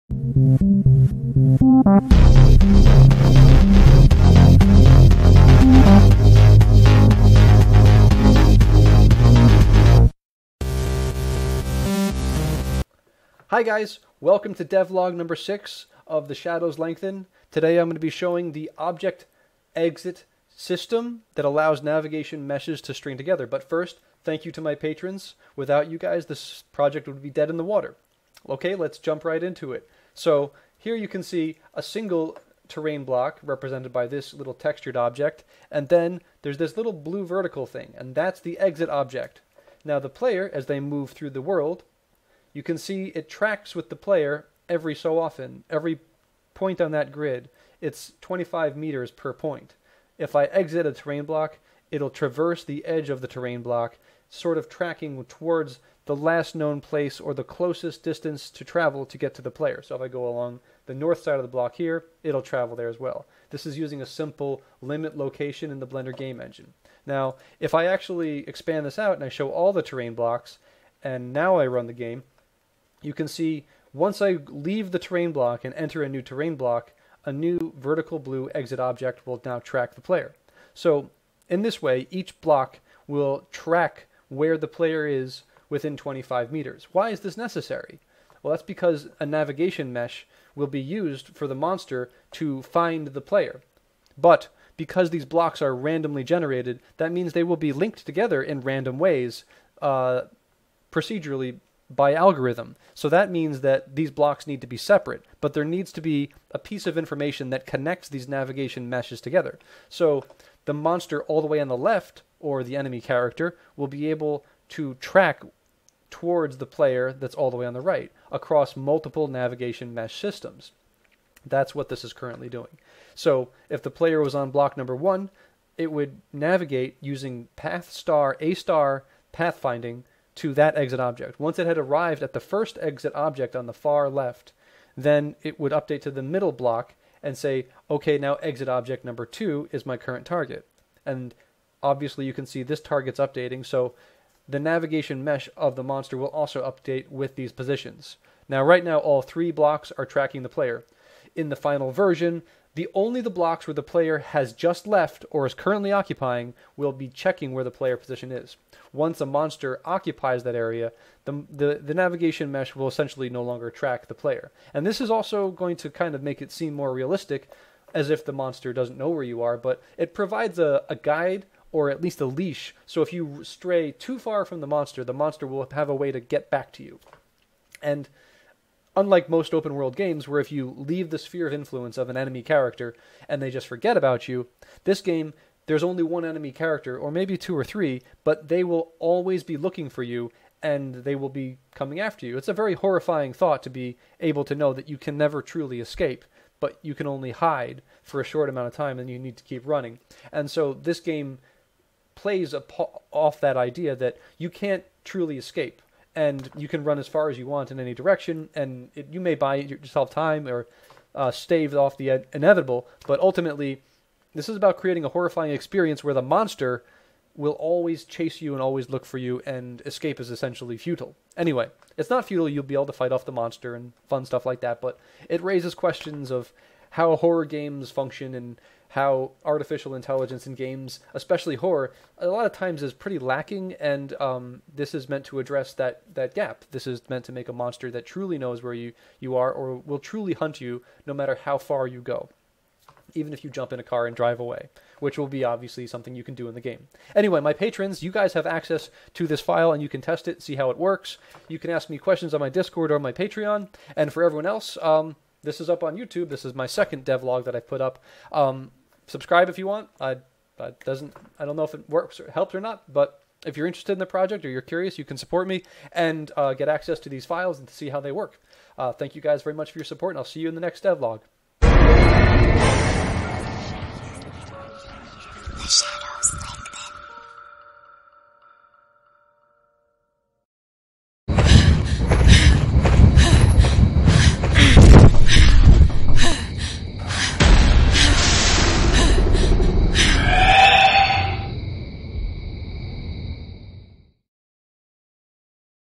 Devlog #6 - AI path-finding for multiple navigation meshes.
This is the terrain tracking system that I have finished coding over the weekend. The blue marker represents the "exit.Object" that is rounded to the nearest 25 meters on the X & Y axis. If the player, seen here as a green dot, travels beyond the domain of the terrain block, then the blue exit.Object is restricted to borders of the terrain block.
In this way, the separate navigation mesh objects will be able to communicate with each other over larger distances.